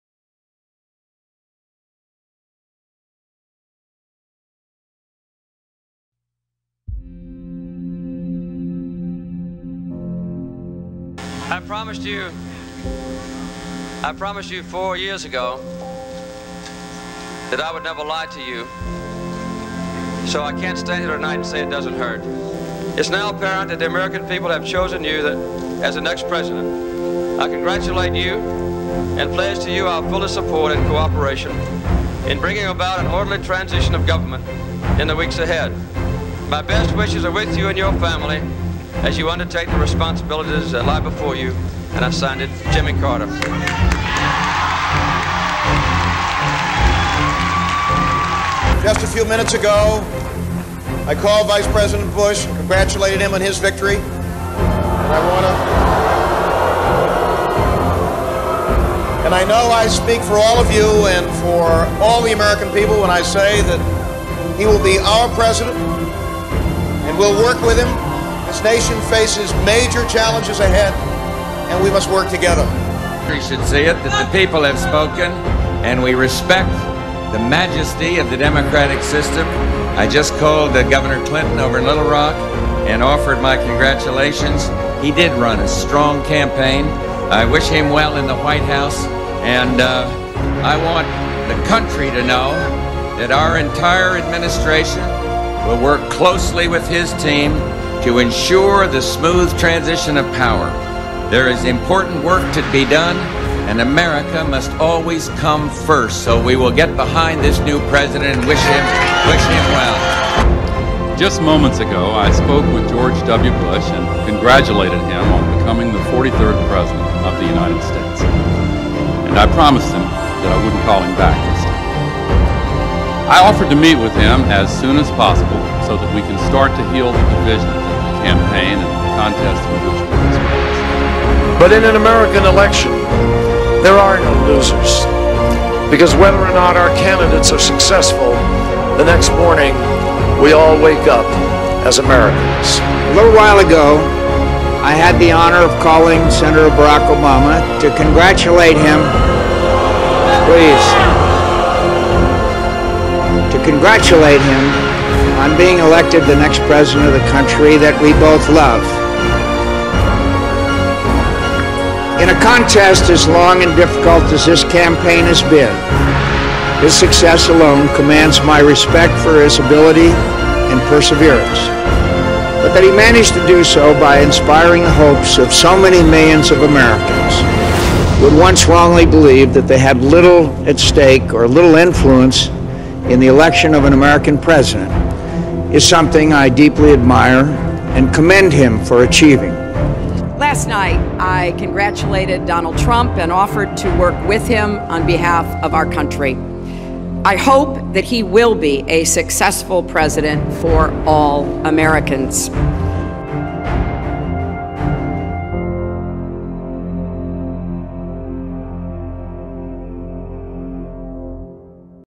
Marco Santiago Filmmaker
A Presidential concession is considered a matter of courtesy and a gracious celebration of American democracy that helps with the peaceful transition of power. Failing to urge one's own supporters toward reconciliation will let embitterment remain between supporters of both candidates, who need to live and work together in the same country for the next four years under a president, whose office term is not fully accepted by almost half of the population.